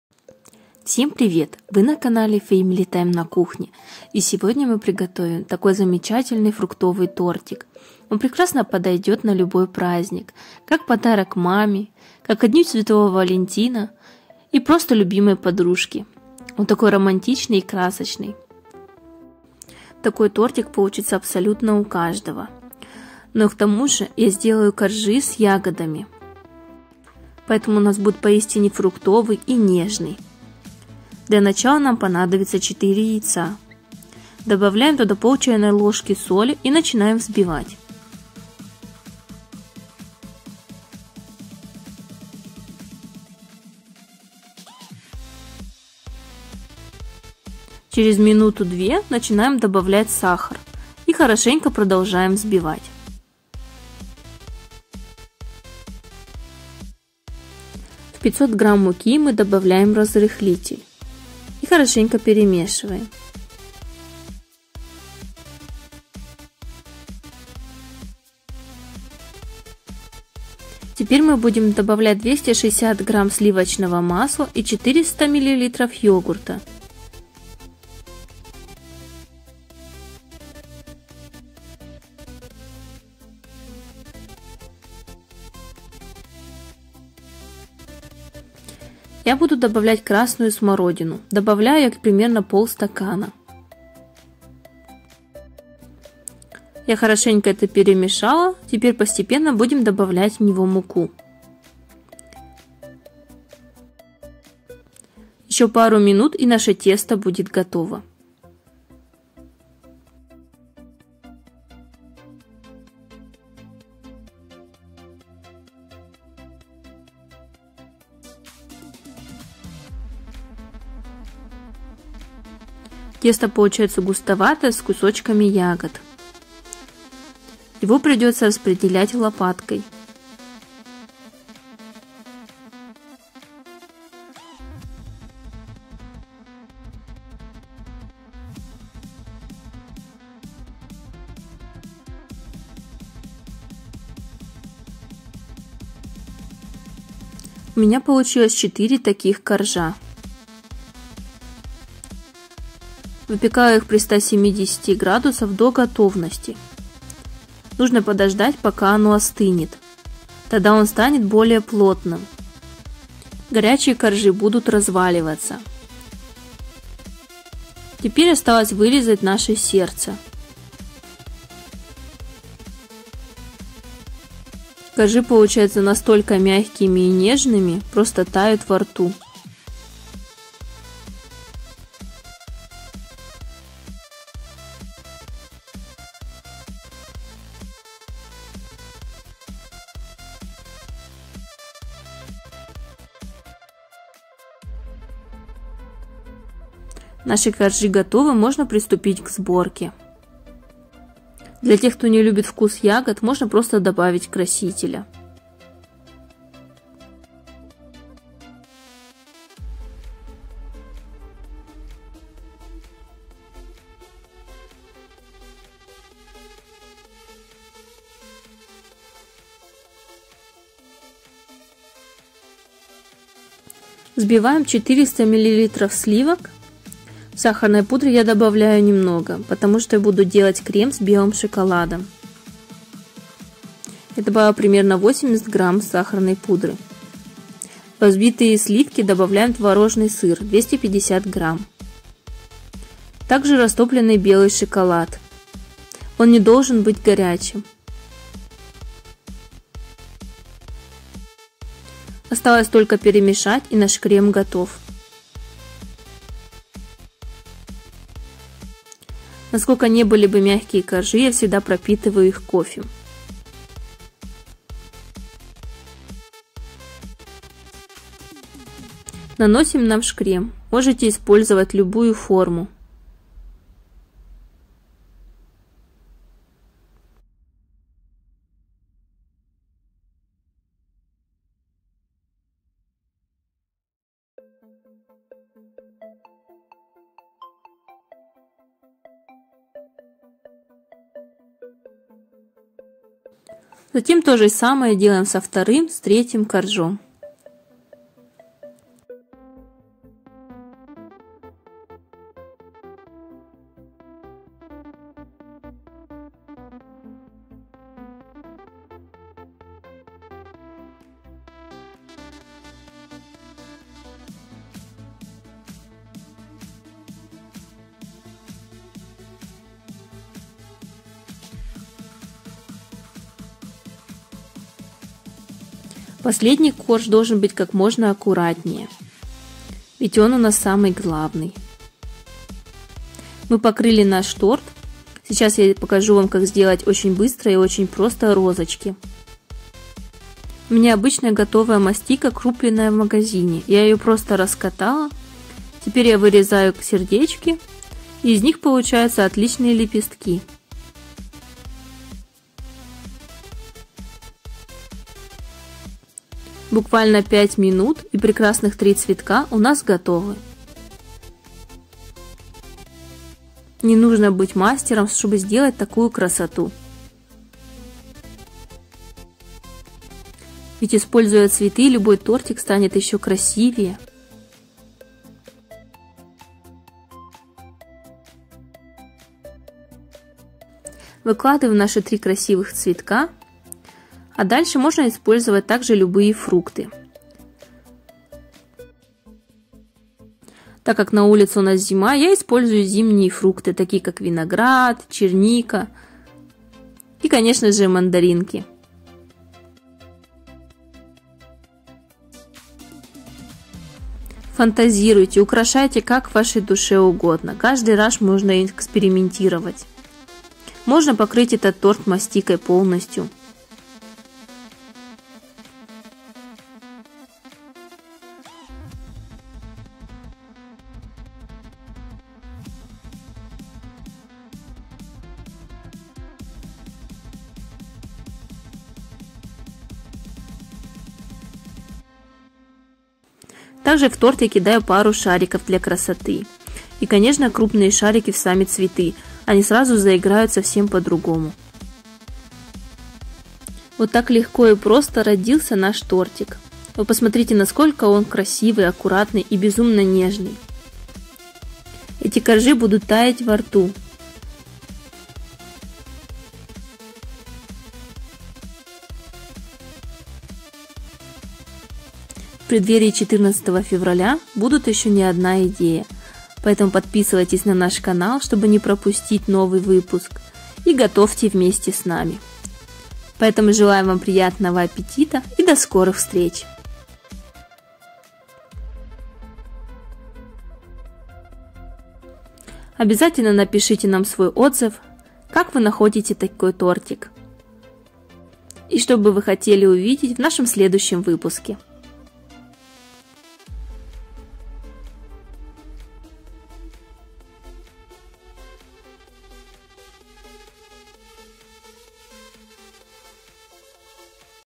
НЕВЕРОЯТНО вкусно и красиво! Фруктовый торт сердце на любой праздник. Торт к 14 февраля
Меня очень сильно вдохновляют праздники, тем более совсем скоро день святого Валентина, а это значит я продолжаю делиться вкусными идеями с романтической тематикой. Сегодня у нас божественный торт с фруктовыми коржами , нежным сливочно шоколадным кремом и зимними фруктами с верху.

Нам понадобится:
400 мл йогурта ( можно заменить на кефир)
260 гр сливочного масла
500 гр муки
сахар 250-300гр
1/2 ч л соли
разрыхлитель
4 яйца
пол стакана ягод ( у меня замороженная смородина)

400мл сливок
80 гр сахарной пудры
1 плитка белого шоколада
250 гр творожного сыра

мастика
виноград двух цветов
черника
мандарины
физалис